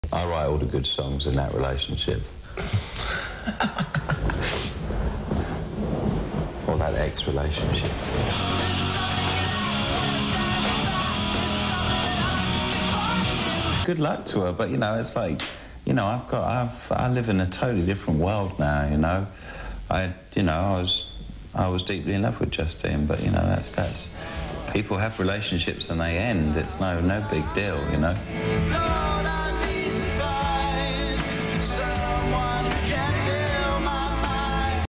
Damon Albarn (Blur) on HIS Influence in Elastica & on Justine Frischmann (00')
Extracted Snippet from a 2000 Justine Interview from Much Music Canada. I thought it would be best to put this up on my channel here just so it reflects over comments Damon mentioned similarly here. Here's Damon on his "influence" that he put upon Elastica & on his Ex-Girlfriend Justine.